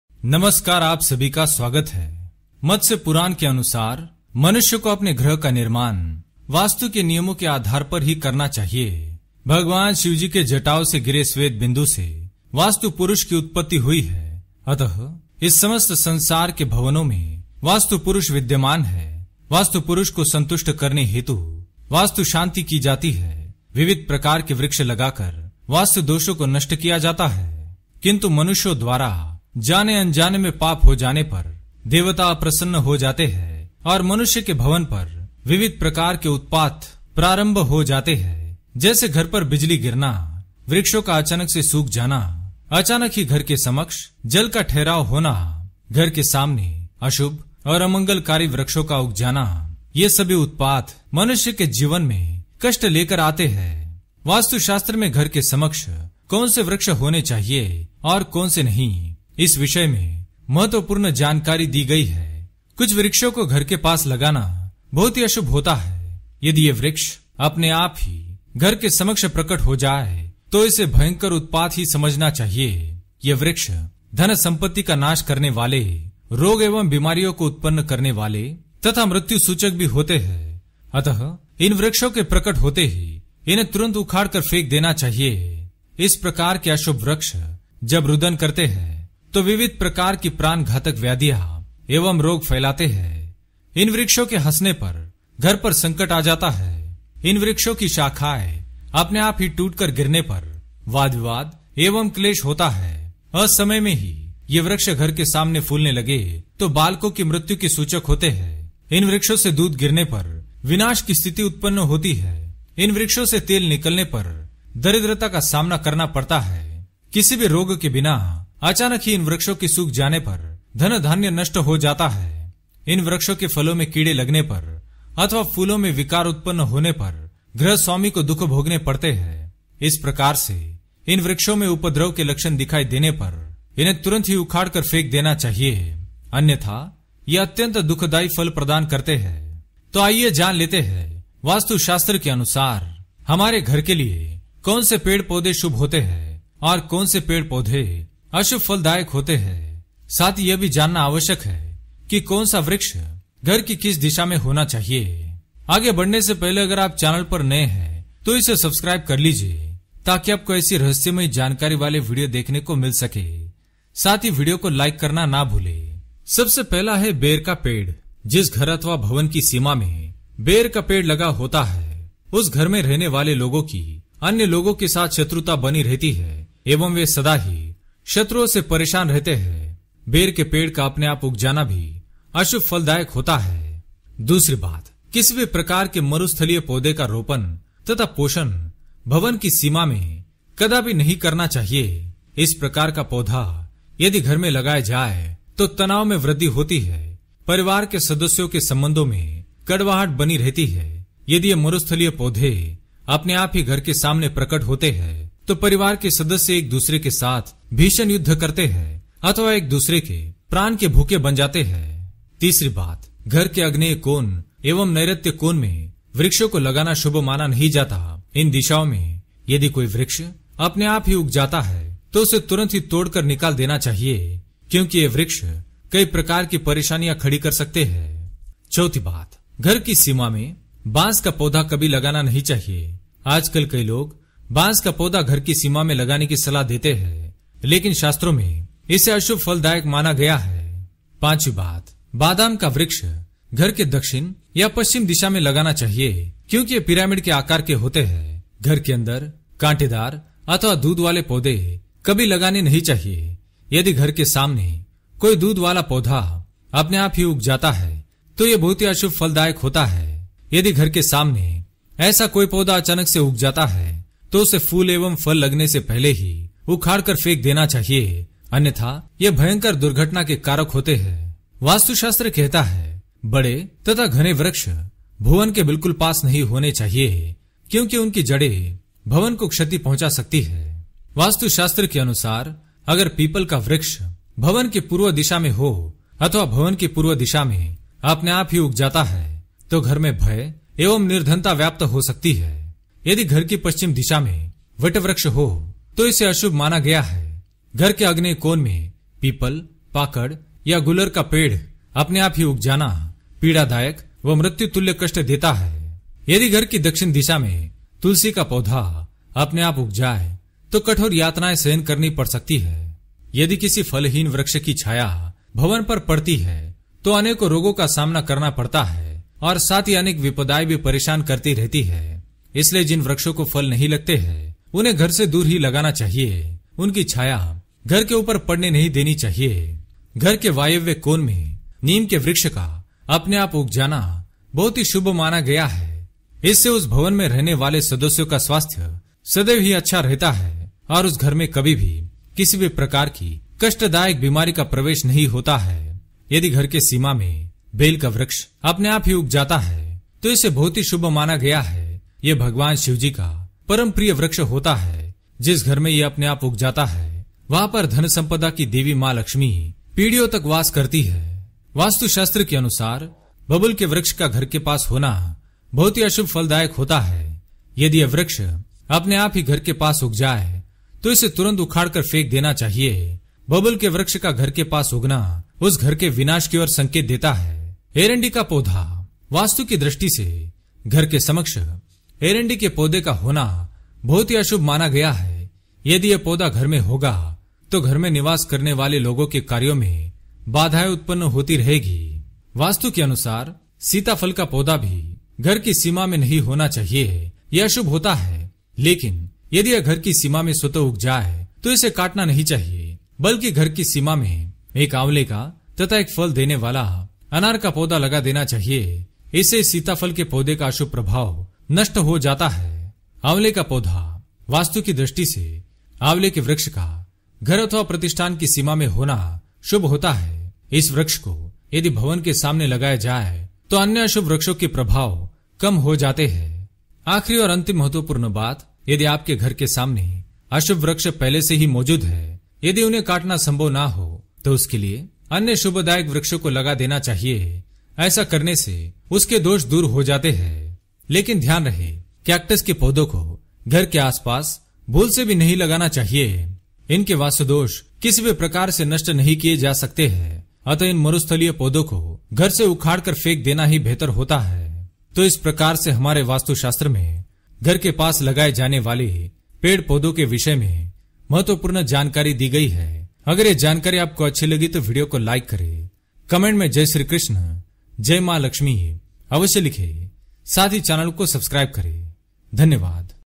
घर के सामने अपने आप उग जाए ये पेड़ तो तुरंत उखाड़कर फेंक दे दुर्भाग्य आता है | vastu tips Krishna
जानिए वास्तुशास्त्र के अनुसार घर के सामने कौन से पेड़ पौधे होने अशुभ होता है, यदि ये वृक्ष घर के सामने अपने आप उग जाए तो तुरंत ही उखाड़कर फेंक देना चाहिए।